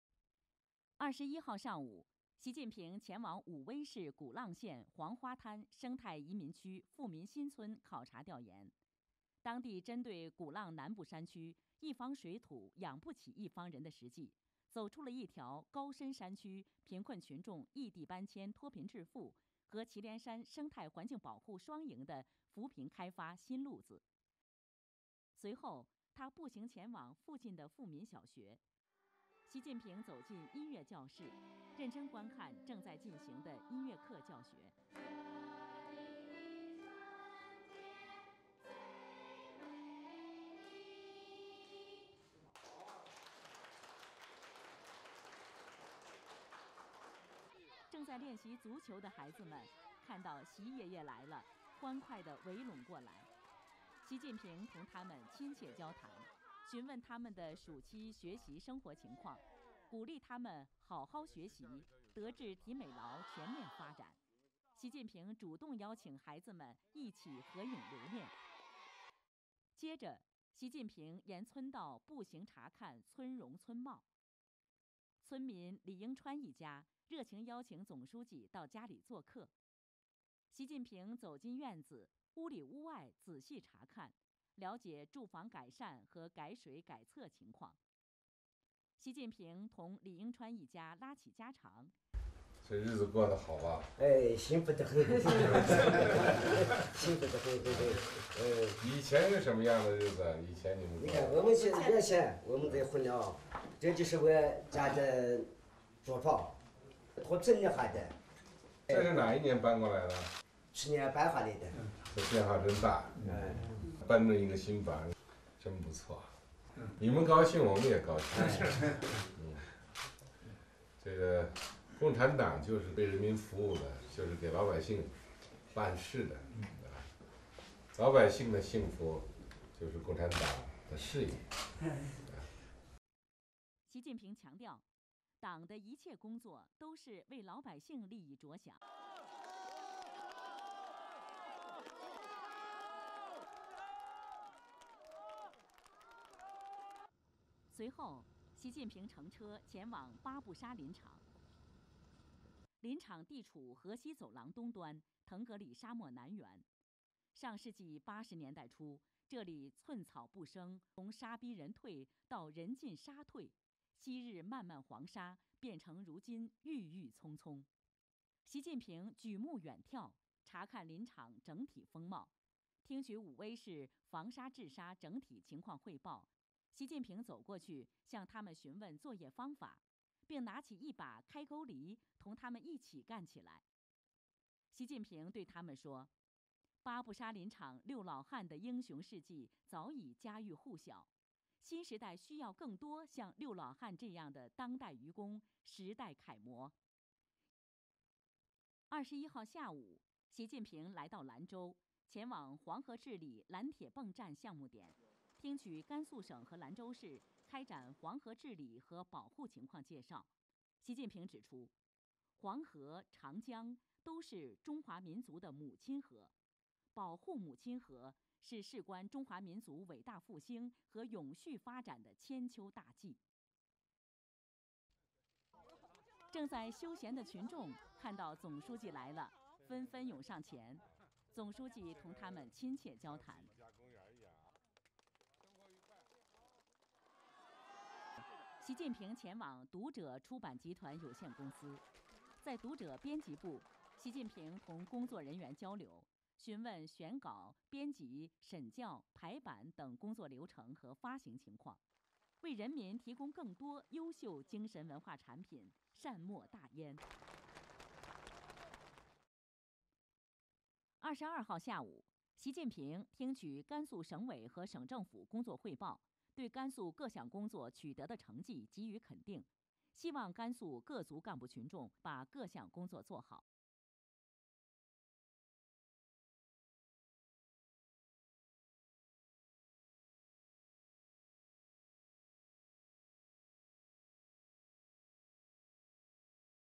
Xi Continues Inspection Tour in Gansu Province
Xi Jinping, general secretary of the Communist Party of China (CPC) Central Committee, visited a rural community and an afforestation area in northwest China's Gansu Province on Wednesday.